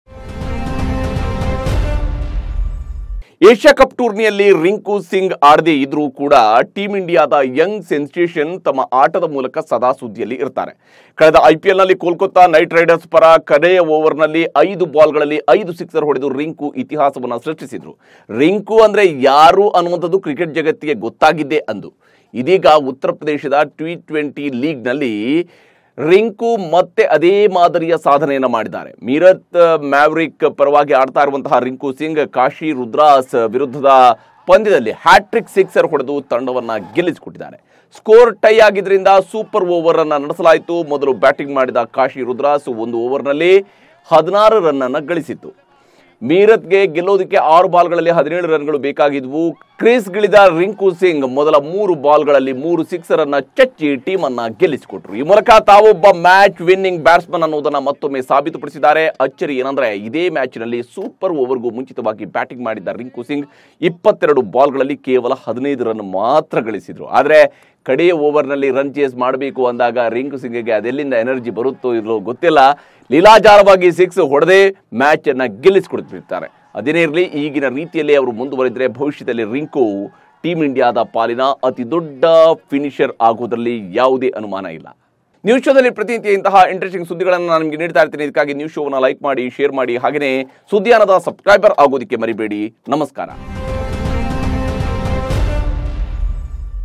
Rinku Singh super over Sixes | ಬ್ಯಾಟಿಂಗ್ ಮೋಡಿಗಾರ ರಿಂಕು | suddiyaana
ಯುಪಿ ಪ್ರೀಮಿಯರ್ ಲೀಗ್ ನಲ್ಲಿ ರಿಂಕು ಸಿಂಗ್ ಭರ್ಜರಿ ಬ್ಯಾಟಿಂಗ್. ರಿಂಕುನ ಚೇಸಿಂಗ್ ನಲ್ಲಿ ಹಿಡಿಯೋರೇ ಇಲ್ಲ ಎಂಬಂತಾಗಿದ್ದು ಹೇಗೆ?